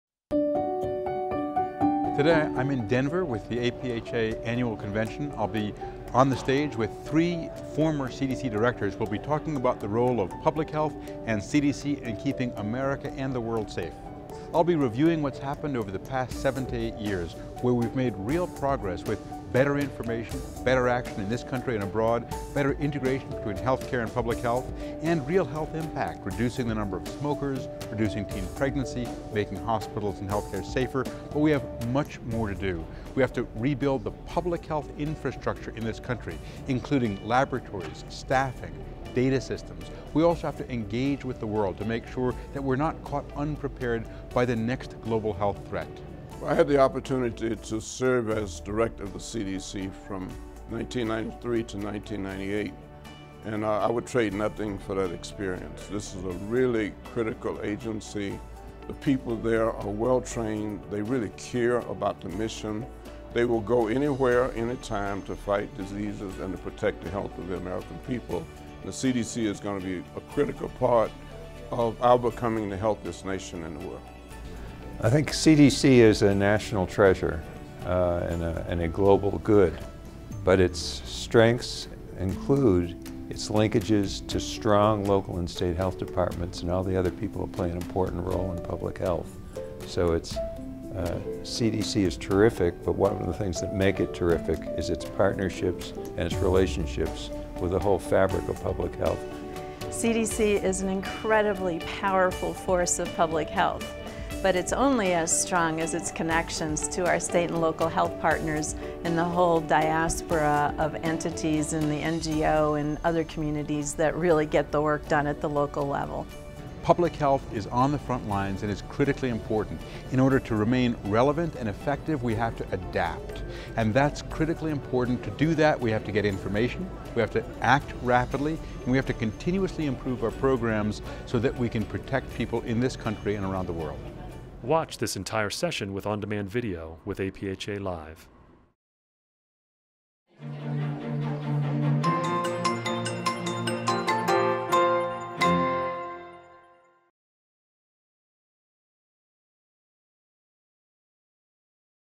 APHA 2016 Monday General Session
APHA TV catches up with Centers for Disease Control and Prevention (CDC) Directors, former and current, as they present at the Monday General Session to the APHA audience.